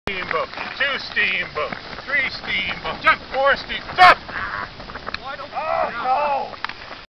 Jim makes a nice pass to a wide open receiver Michael? at Super Party XIV
With the steamboat dead count quickly approaching Jim finds a wide open receiver and throws a laser for the touchdown.